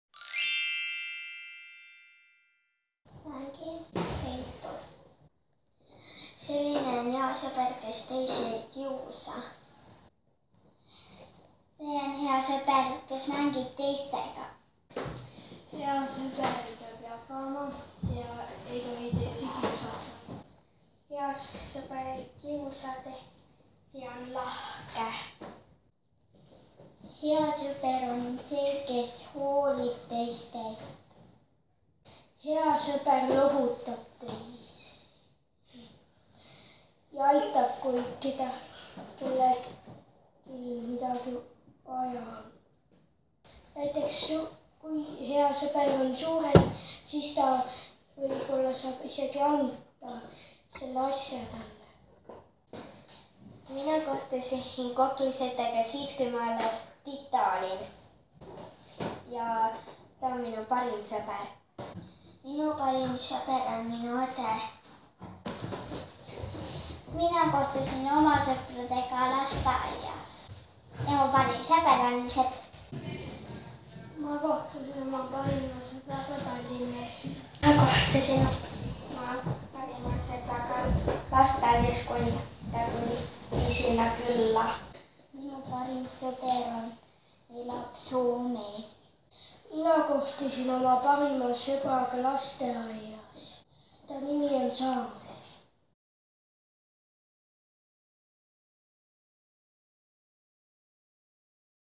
Sõbrapäev
Väikesed meediaringi liikmed arutavad sõpruse ja sõprade üle.